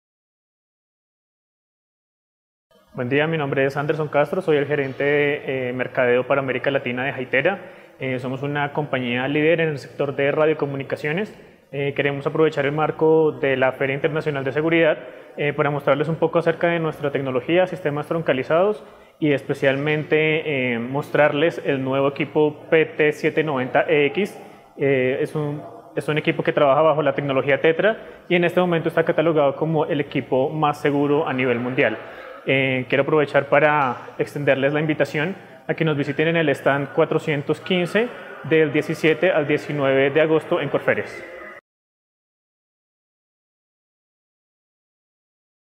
Conozca las novedades que encontrará en la Feria Internacional de Seguridad 2016
Feria Internacional de Seguridad, del 17 al 19 de agosto en Corferias.